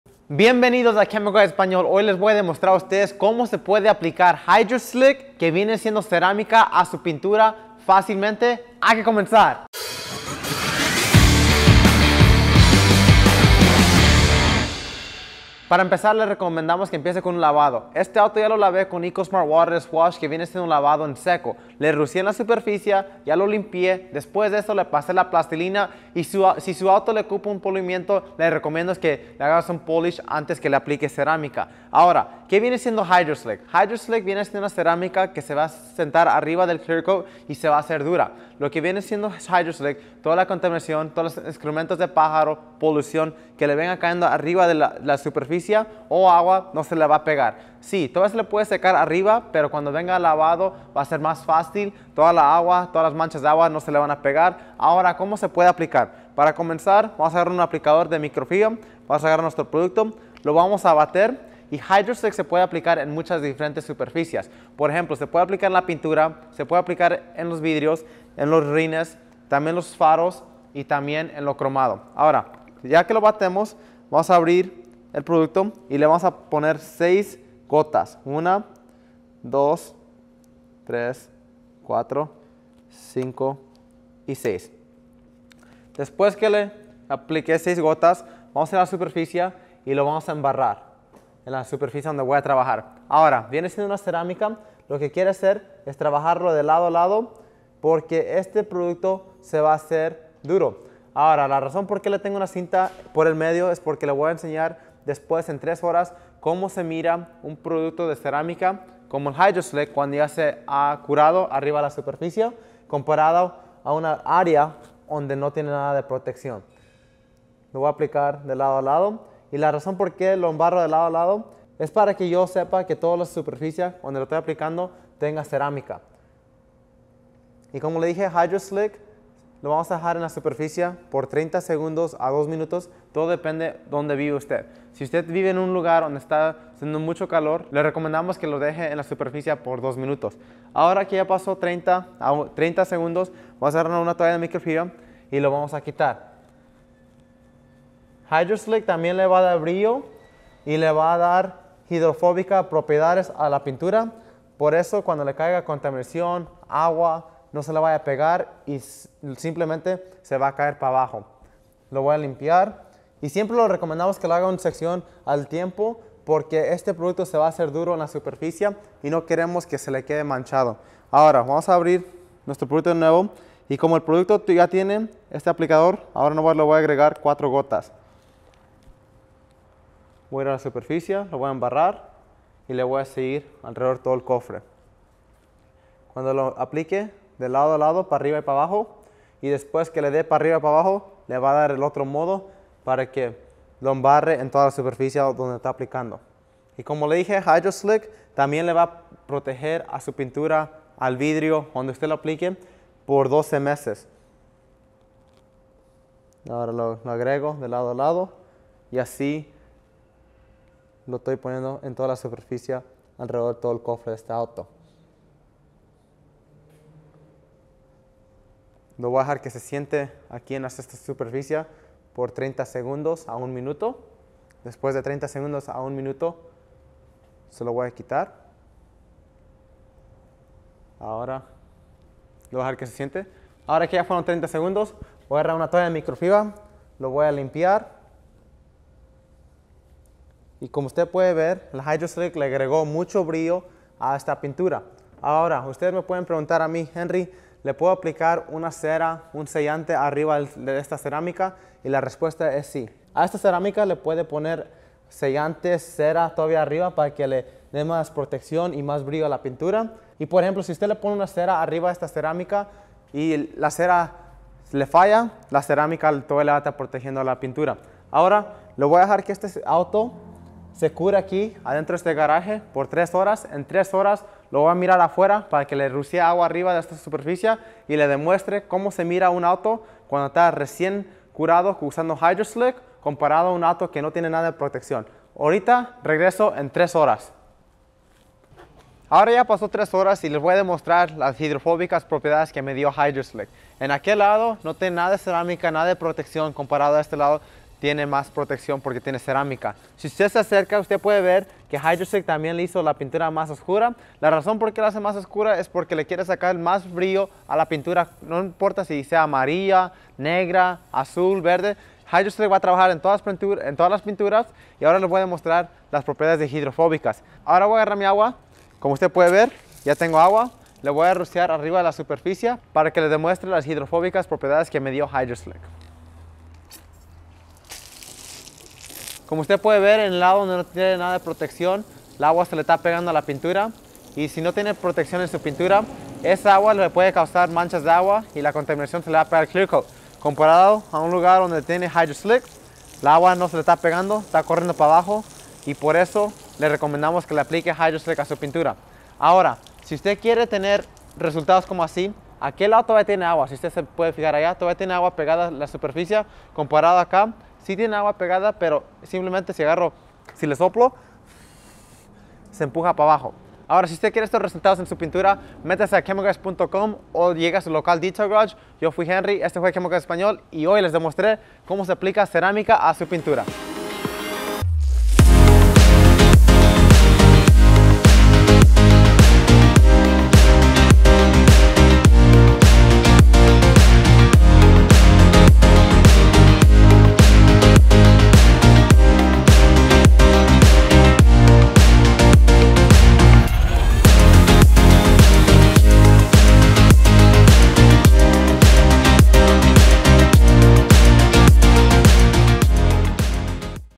Cómo Aplicar Cerámica A La Pintura Del Automóvil - Chemical Guys
En este video, le mostraremos cómo aplicar cerámica a su pintura con HydroSlick. Para empezar te recomendamos que laves el coche y le pases la arcilla. Después de eso, le recomendamos que comienzan a aplicar HydroSlick. HydroSlick es un verdadero recubrimiento cerámico que brinda una protección inigualable, brillo brillante y claridad óptica en un gel cerámico fácil de aplicar.Vamos a empezar aplicando 6 gotas del producto sobre la aplicación de microfibras. Luego lo aplicamos a la superficie. Vamos a dejar reposar el producto en la superficie durante 30 segundos y luego lo vamos a retirar. Después de 3 horas te mostramos cómo la superficie se vuelve hidrofóbica. El agua no se pega a la pintura y estos reducen las manchas de agua.